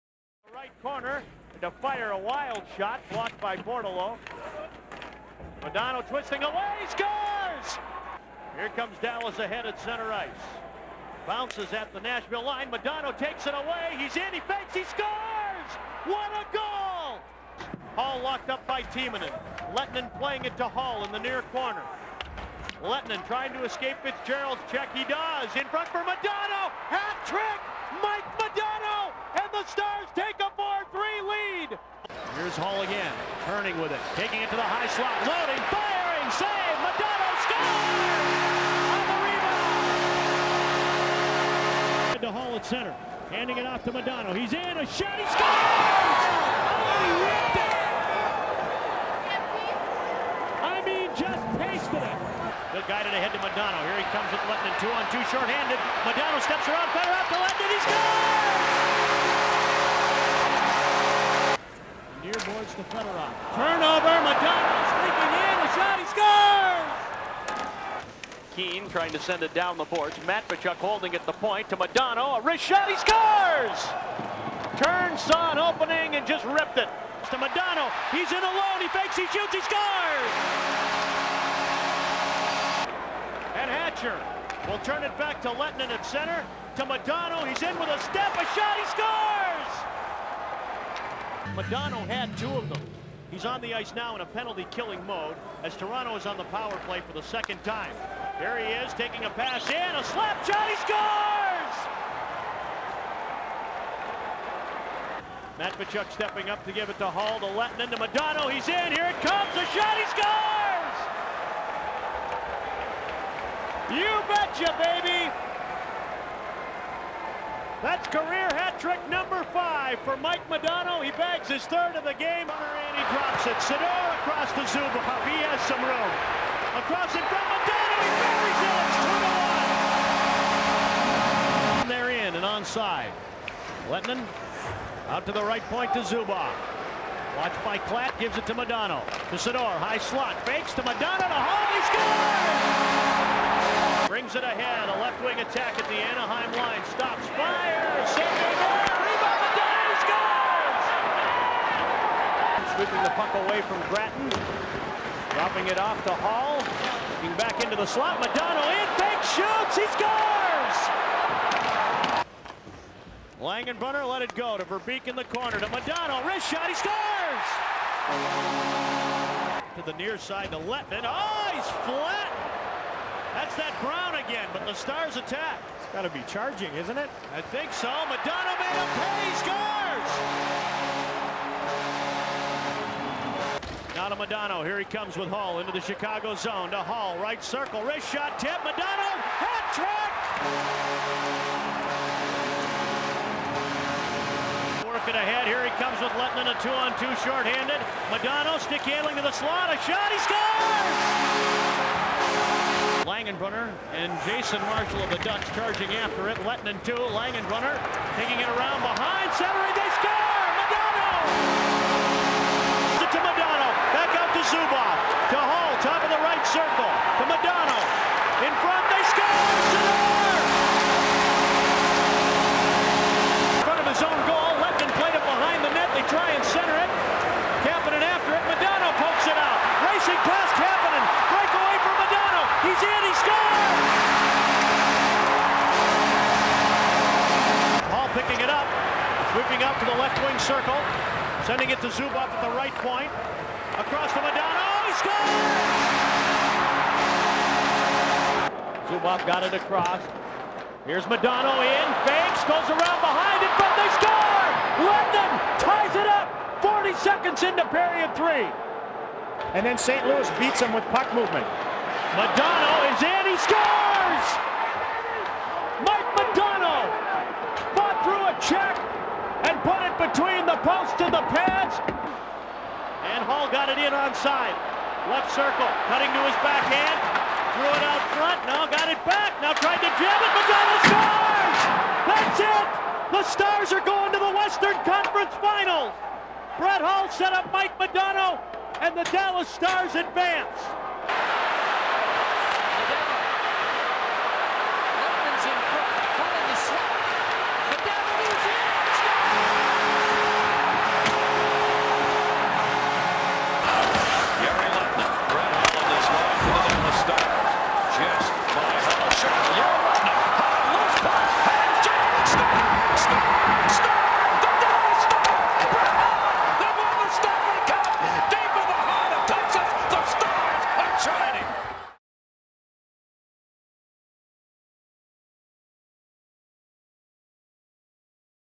Mike Modano Highlights 1998 - 1999
This is a little highlight video of mike modano. mike modano will forever be my favorite dallas star and my all time favorite player. Wish he would have never retired but all good players have to retire at some point in life.